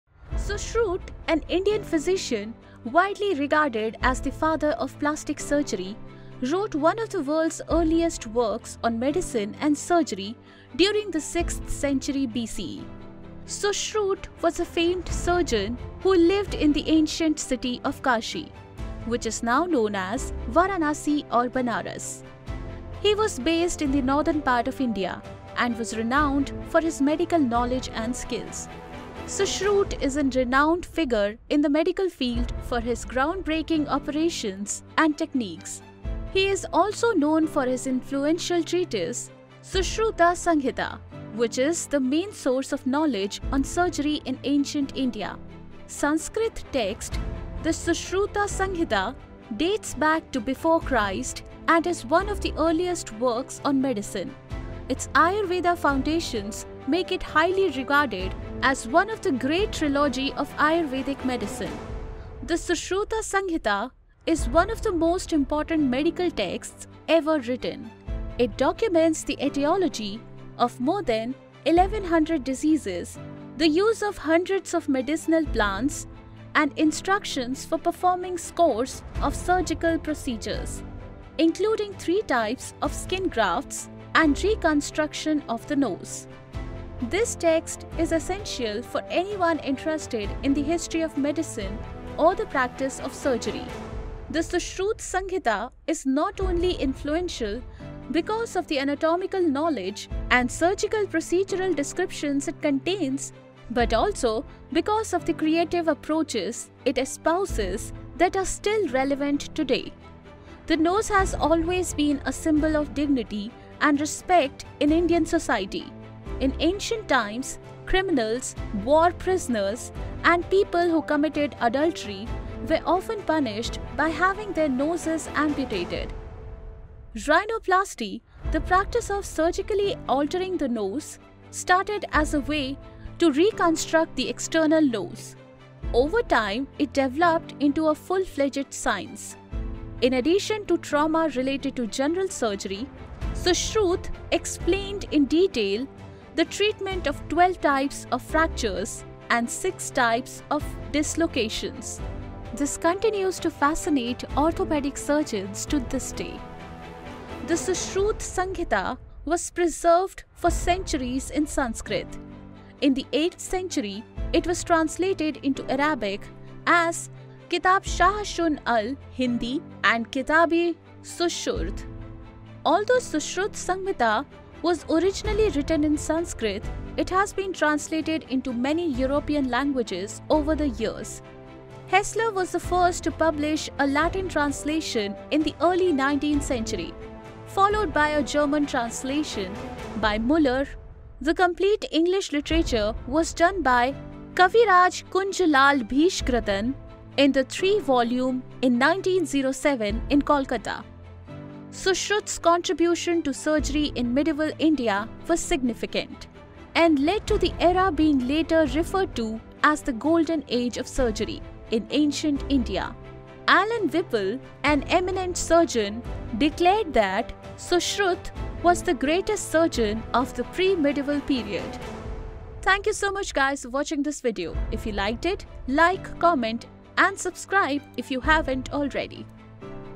Sushruta: Father Of Cosmetic Surgery
Sushruta, an Indian physician widely considered the "father of plastic surgery," wrote one of the world's earliest works on medicine and surgery during the 6th century BC. Sushruta was a famous surgeon who lived in the ancient city of Kashi, which is now known as Varanasi or Benaras. He was based in the northern part of India and was known for his medical knowledge and skills.